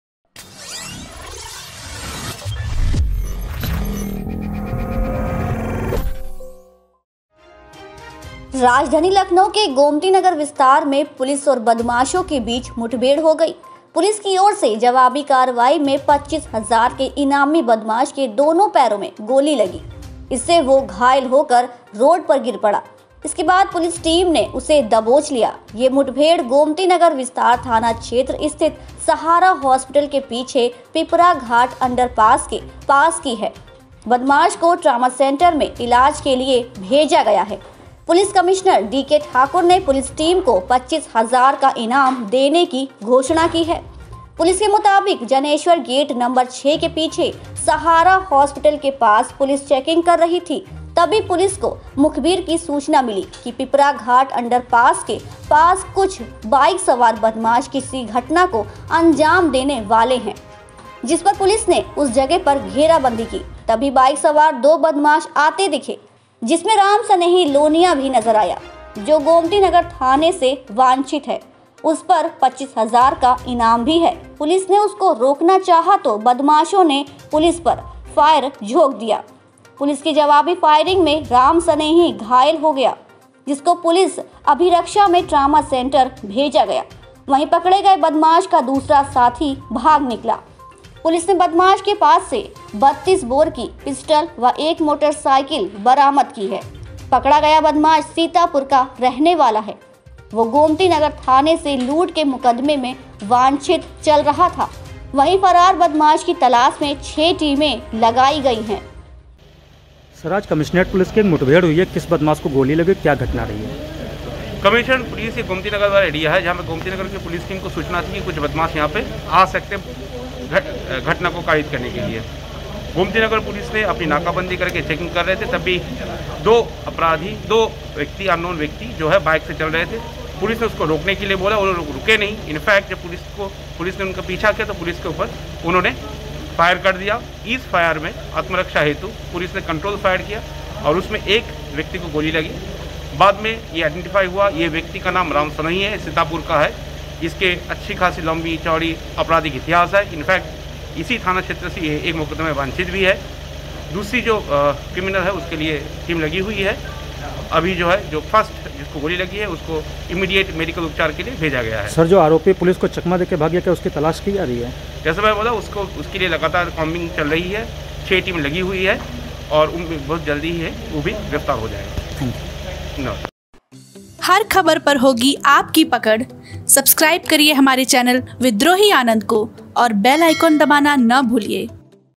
Lucknow में मुठभेड़ : 25 हजार के इनामी बदमाश को Police ने धर दबोचा
राजधानी लखनऊ के गोमती विस्तार में पुलिस और बदमाशों के बीच मुठभेड़ हो गई। पुलिस की ओर से जवाबी कार्रवाई में 25 हजार के इनामी बदमाश के दोनों पैरों में गोली लगी, इससे वह घायल होकर रोड पर गिर पड़ा। इसके बाद पुलिस टीम ने उसे दबोच लिया। यह मुठभेड़ गोमतीनगर विस्तार थाना क्षेत्र स्थित सहारा हॉस्पिटल के पीछे पिपरा घाट अंडर पास के पास की है। बदमाश को ट्रामा सेंटर में इलाज के लिए भेजा गया है। पुलिस कमिश्नर डीके ठाकुर ने पुलिस टीम को 25 हजार का इनाम देने की घोषणा की है।
पुलिस के मुताबिक, जनेश्वर गेट नंबर 6 के पीछे सहारा हॉस्पिटल के पास पुलिस चेकिंग कर रही थी। तभी पुलिस को मुखबिर की सूचना मिली कि पिपरा घाट अंडरपास के पास कुछ बाइक सवार बदमाश किसी घटना को अंजाम देने वाले हैं। जिस पर पुलिस ने उस जगह पर घेराबंदी की। तभी बाइक सवार दो बदमाश आते दिखे, जिसमें रामसनेही लोनिया भी नजर आया, जो गोमतीनगर थाने से वांछित है और उस पर 25 हजार का इनाम भी है। पुलिस ने उसको रोकना चाहा तो बदमाशों ने पुलिस पर फायर झोंक दिया।
पुलिस की जवाबी फायरिंग में रामसनेही घायल हो गया। जिसको पुलिस अभिरक्षा में ट्रामा सेंटर भेजा गया। वहीं, पकड़े गए बदमाश का दूसरा साथी भाग निकला। पुलिस ने बदमाश के पास से 32 बोर की पिस्टल व एक मोटरसाइकिल बरामद की है। पकड़ा गया बदमाश सीतापुर का रहने वाला है। वह गोमतीनगर थाने से लूट के मुकदमे में वांछित चल रहा था। वहीं, फरार बदमाश की तलाश में 6 टीमें लगाई गई हैं।